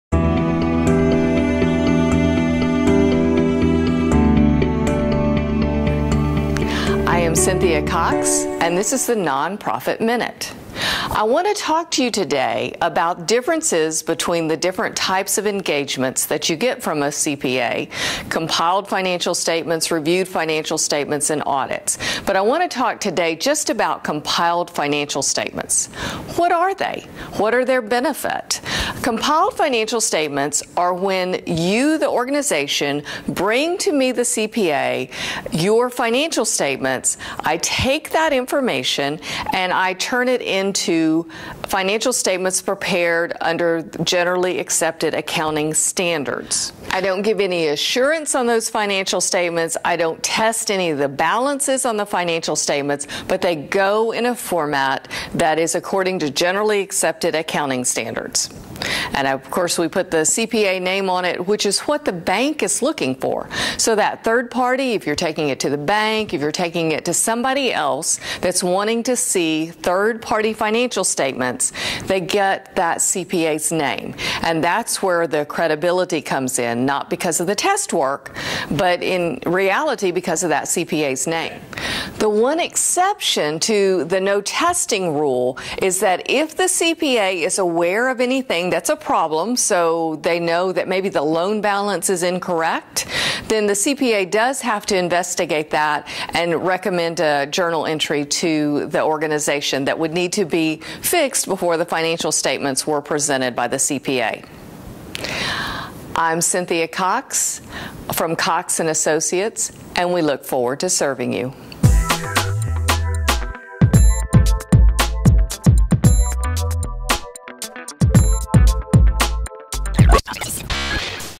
The Nonprofit Minute - Cynthia Cox - Compilations
In this episode of The Nonprofit Minute Cynthia Cox (Owner, Cox and Associates CPAs) outlines the elements and goals of Compilations. This type of engagement is the least intensive of accounting services, followed by Reviews and Audits.